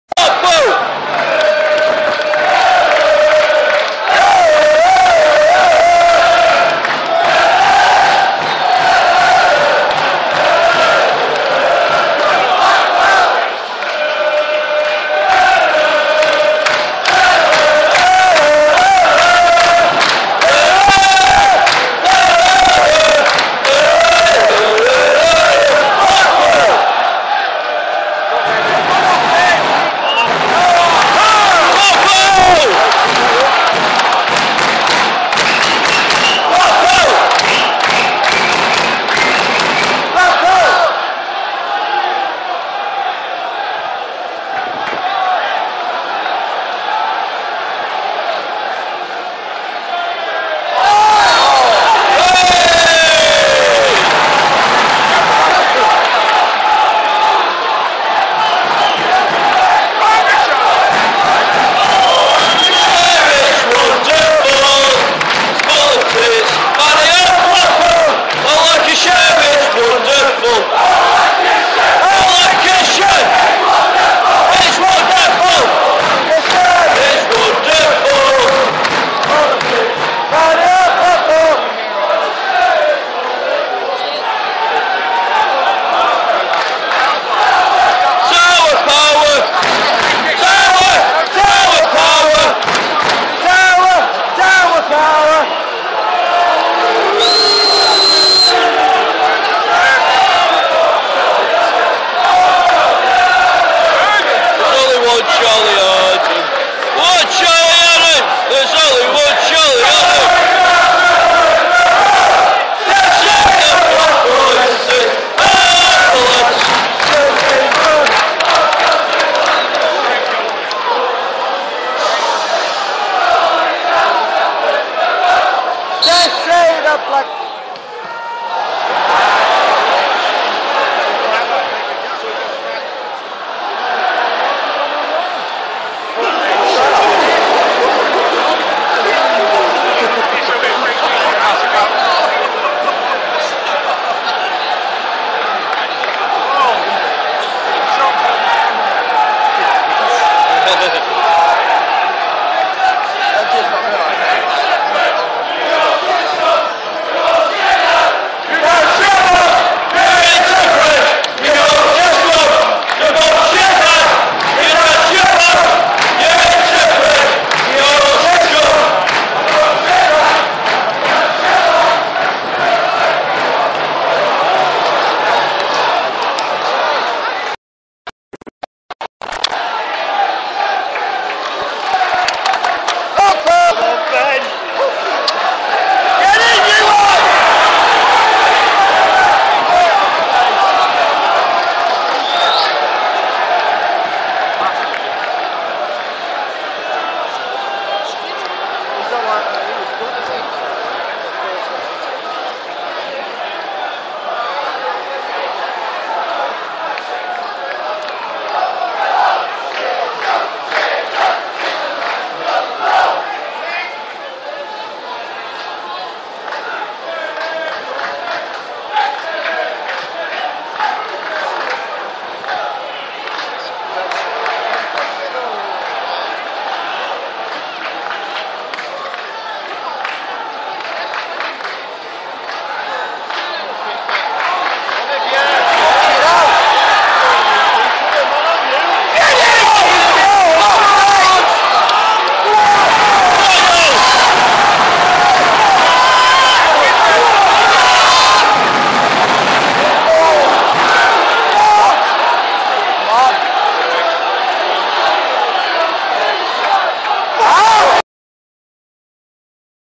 Blackpool vs Sheffield Wednesday 19th January 2010
Blackpool 1:2 Sheffield Wednesday
Neil Eardley Freekick